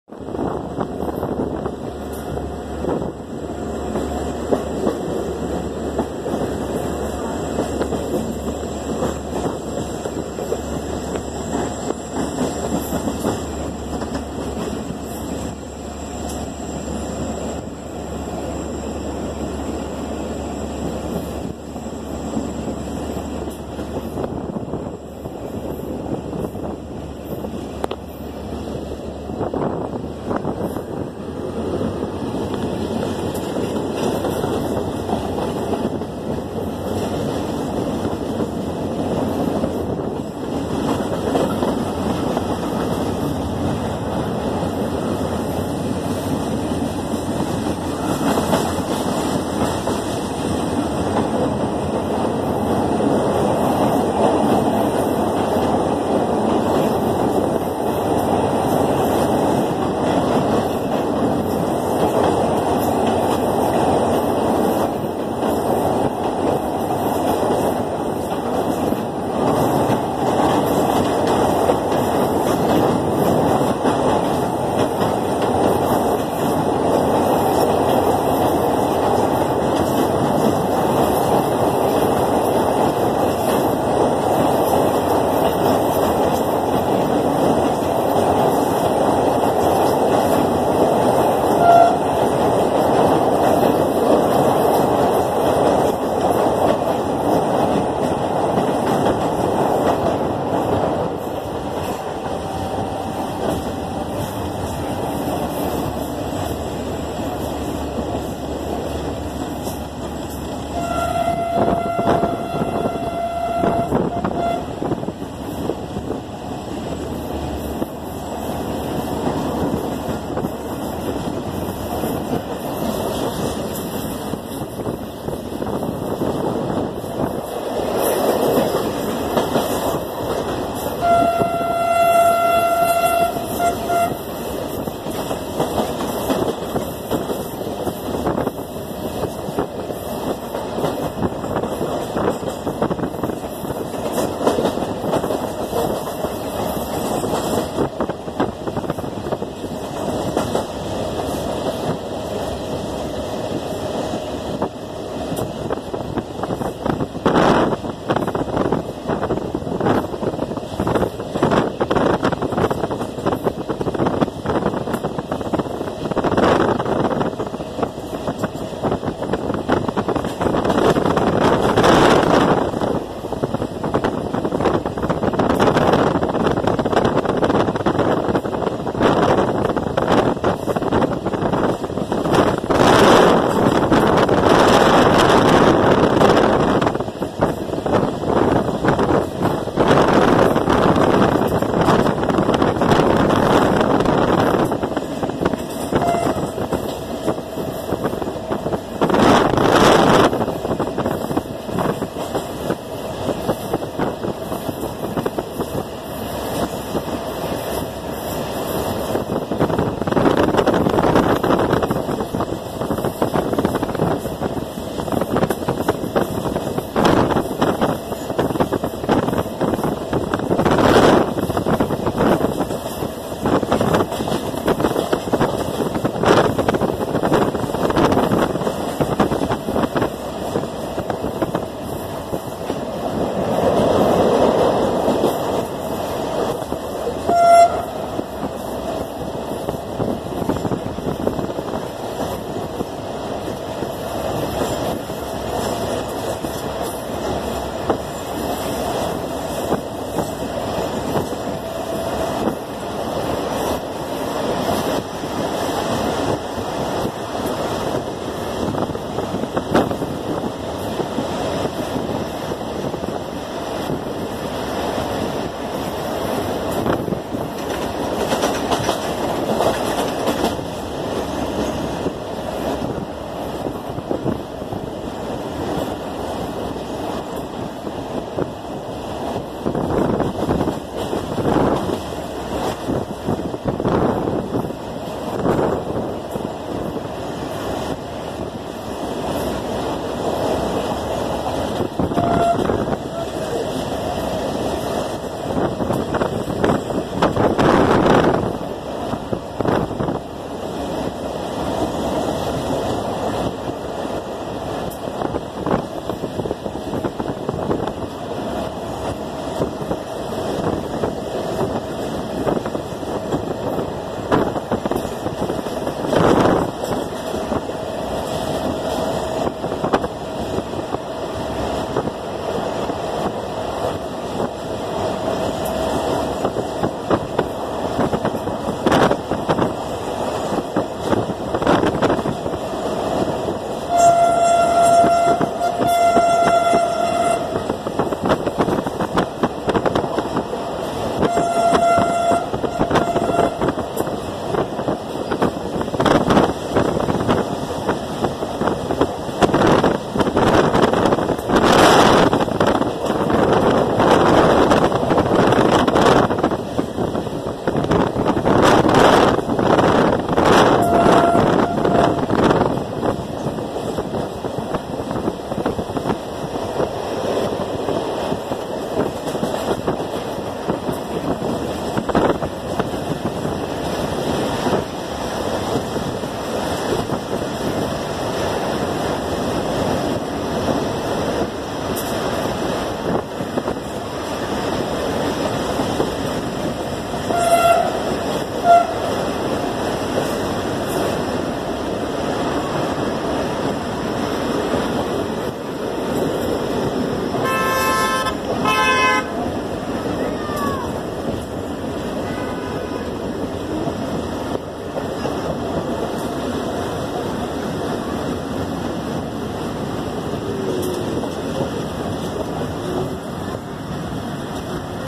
Pakistan Express Mehran Express beautiful crossing Kotri River Railway Bridge
On 25 July 2018 149 up Mehran Express cross Kotri Railway Bridge ..Very beautiful and greenery view ...Lovely track sound and cloudly weather so much pleasure ...